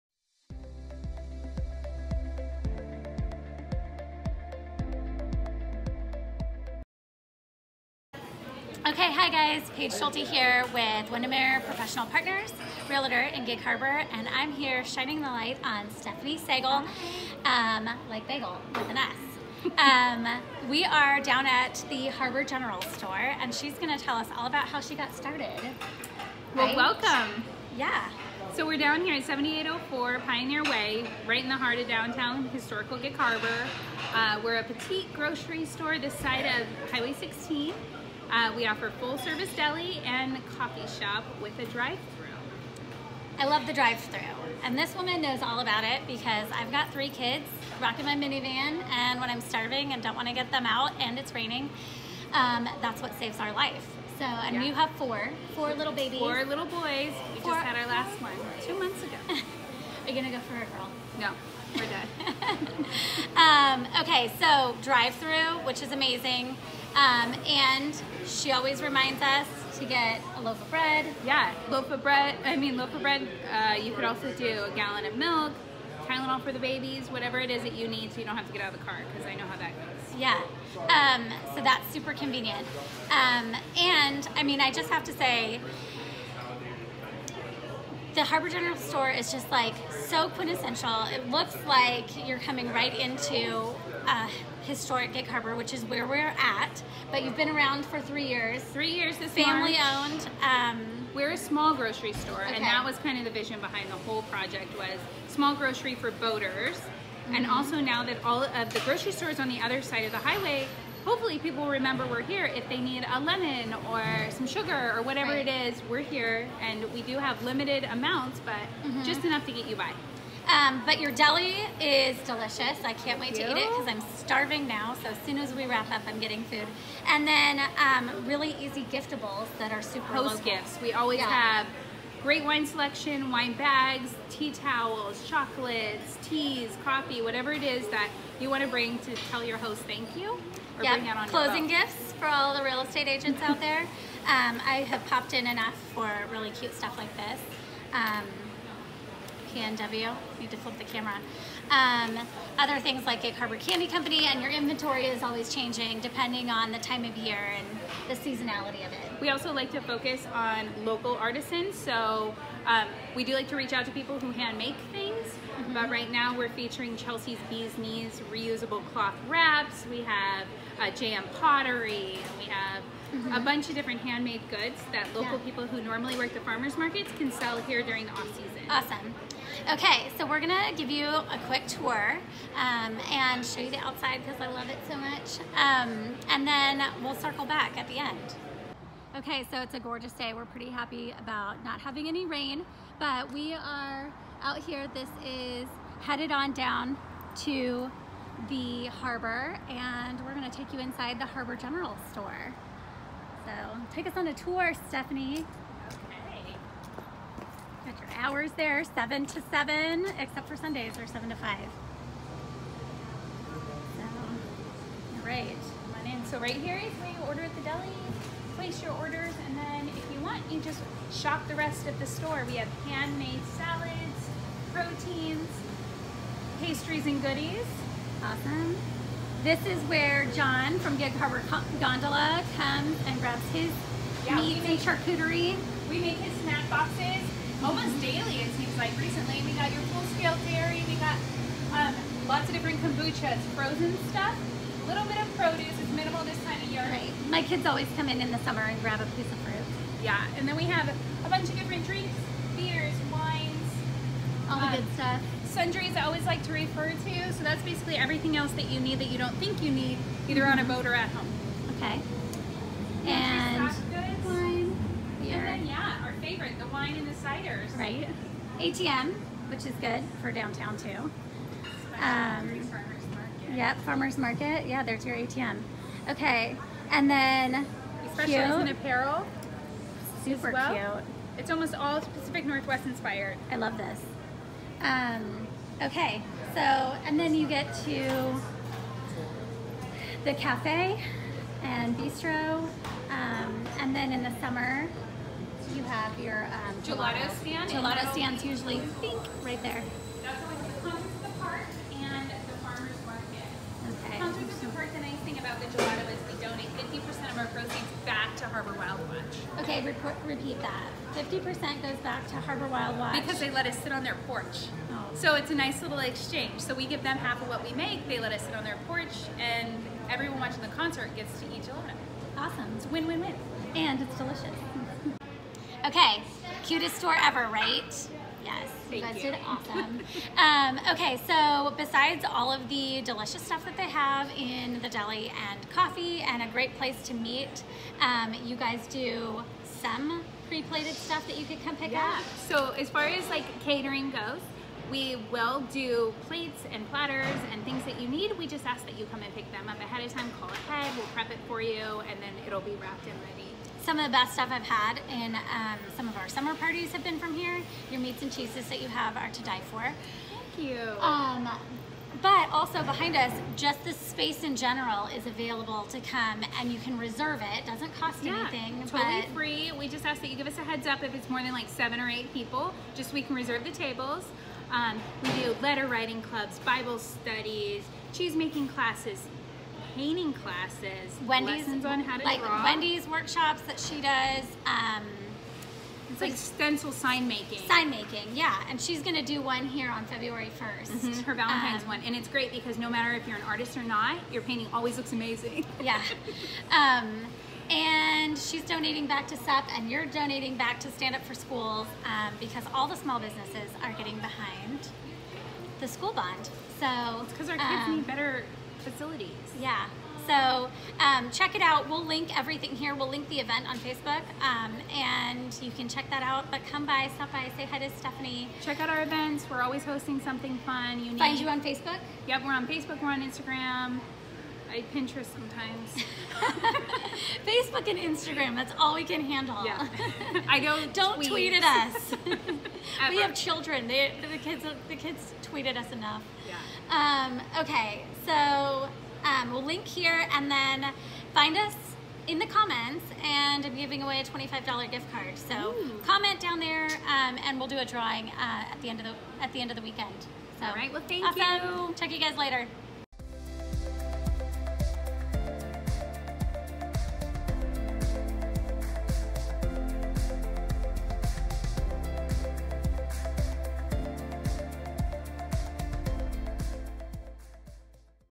BizintheGig: Harbor General Store
All about Gig Harbors favorite grocery store and cafe, the Harbor General Store is an iconic storefront in Historic Downtown Gig Harbor. Learn all about what this store offers, from delish coffee, lunch, gelato to groceries and hostess gifts- there's so much to love!
Paige Schulte, Realtor
2727 Hollycroft St #210
Gig Harbor, WA 98335